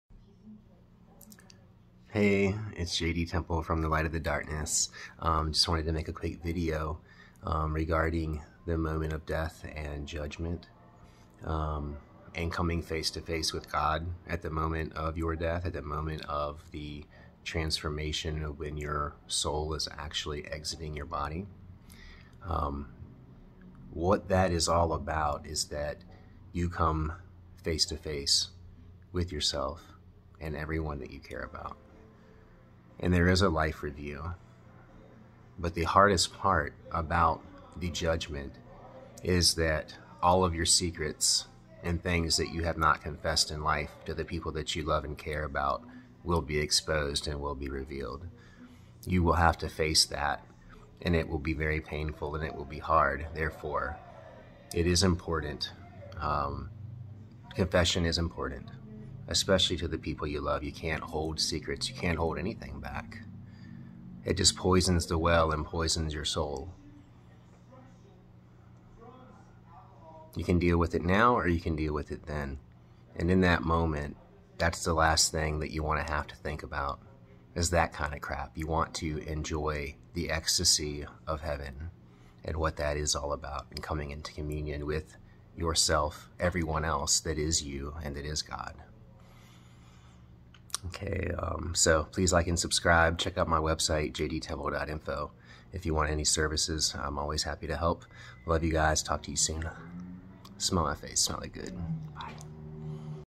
Judgement at the Moment of Death
There is a life review and revealing of all things at the moment of death. You must come face to face with it All.